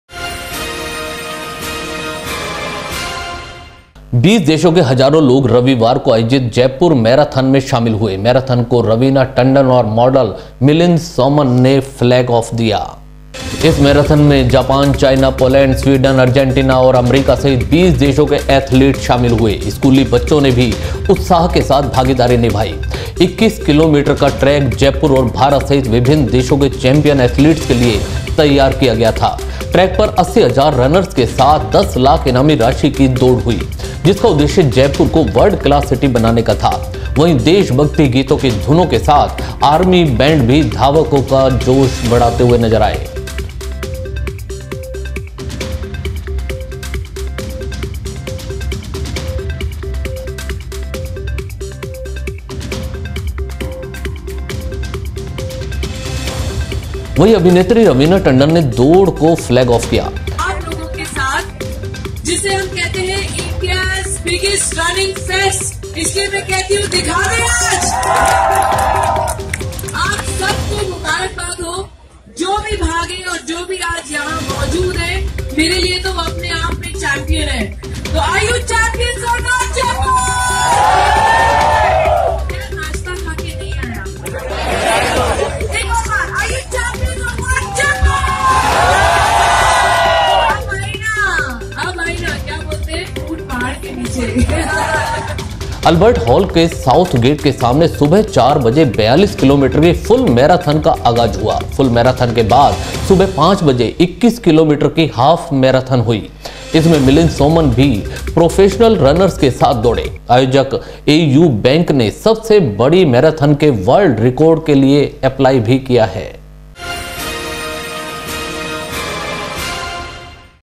जयपुर - मैराथन में दौड़े 20 देशों के हजारों धावक, स्कूली बच्चों ने भी दिखाया जोश
बीस देशों के हजारों लोग रविवार को आयोजित जयपुर मैराथन में शामिल हुए। मैराथन को रवीना टंडन और मॉडल मिलिंद सोमन ने फ्लैग ऑफ किया।
 इस मैराथन में जापान, चाइना, पौलेंड, स्वीडन, अर्जेंटीना, और अमेरिका सहित 20 देशों के एथलीट शामिल हुए। स्कूली बच्चों ने भी उत्साह के साथ भागीदारी निभाई। 21 किलोमीटर का ट्रैक जयपुर और भारत सहित 20 देशों के चैम्पियन एथलीट्स के लिए तैयार किया गया था। ट्रैक पर 80 हजार रनर्स के साथ 10 लाख इनामी राशि की दौड़ हुई, जिसका उद्देश्य जयपुर को वल्र्ड क्लास सिटी बनाने का था। वहीं देशभक्ति गीतों की धुनों के साथ आर्मी बैंड भी धावकों का जोश बढाता नजर आया।
 वहीं, अभिनेत्री रवीना टंडन ने दौड़ को फ्लैग ऑफ किया। अल्बर्ट हाल के साउथ गेट के सामने सुबह 4 बजे 42 किलोमीटर की फुल मैराथन का आगाज हुआ। फुल मैराथन के बाद सुबह 5 बजे 21 किलोमीटर की हाफ मैराथन शुरू हुई। इसमें मिलिंद सोमन भी प्रोफेशनल रनर्स के साथ दौड़े। आयोजक ए.यू. बैंक ने सबसे बड़ी मैराथन के वल्र्ड रिकॉर्ड के लिए एप्लाई भी किया है।